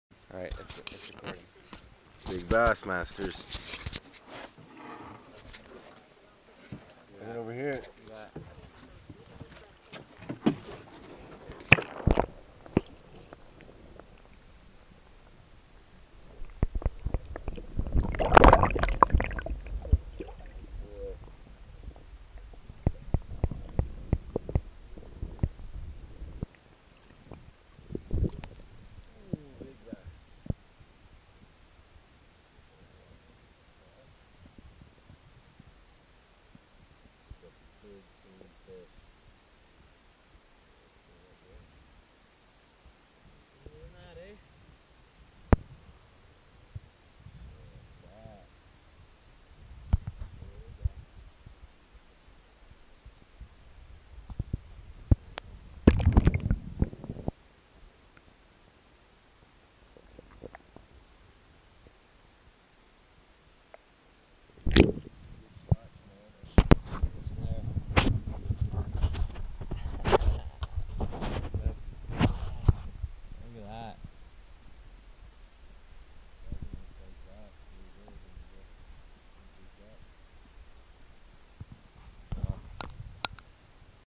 Hoola Popper bass catch- CanadianOutdoors!
Caught a good little bass with the trusty hoolapopper, never go wrong with that lure!
- CanadianOutdoors!